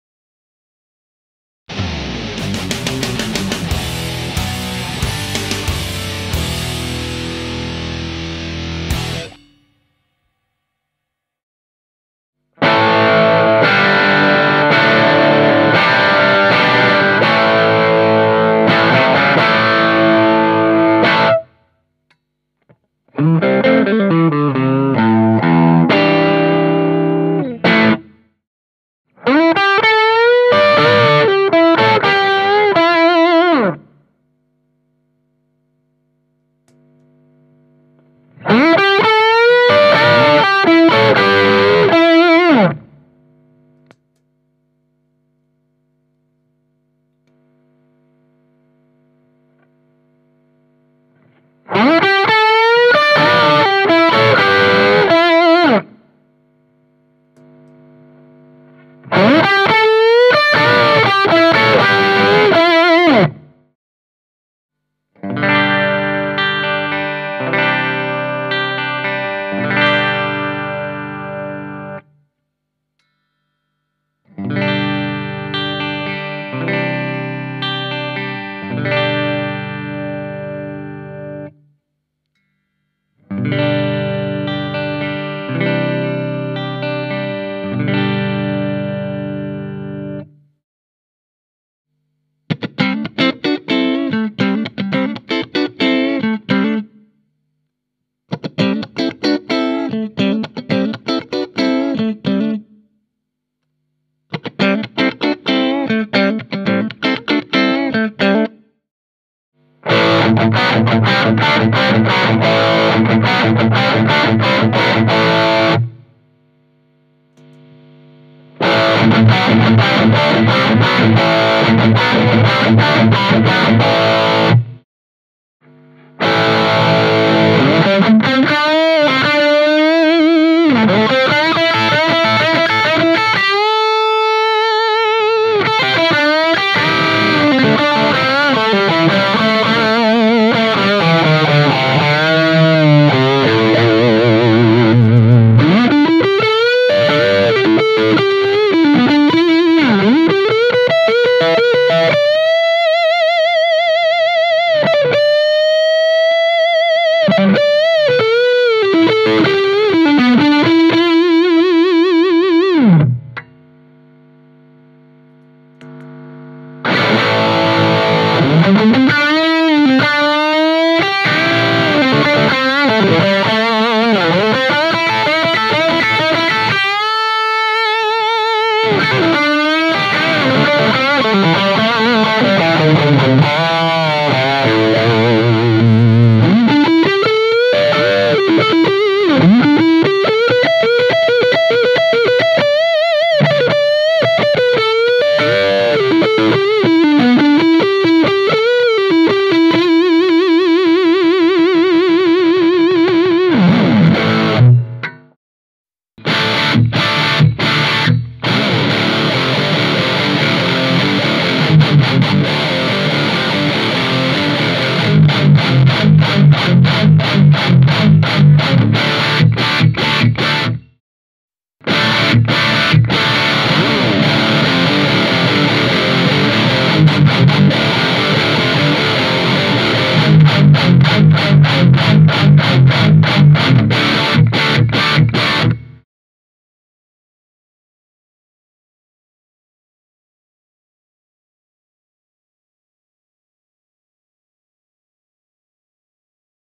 Bogner Ecstasy 101B - Celestion V30 - Les Paul - Sound Test - Demo
Gear:
Bogner Ecstasy 101B
Les Paul Gallinaro Lindy Fralin Pickups
Fender Stratocaster Texas Special Pickups
2x12 Celestion V 30
Shure SM57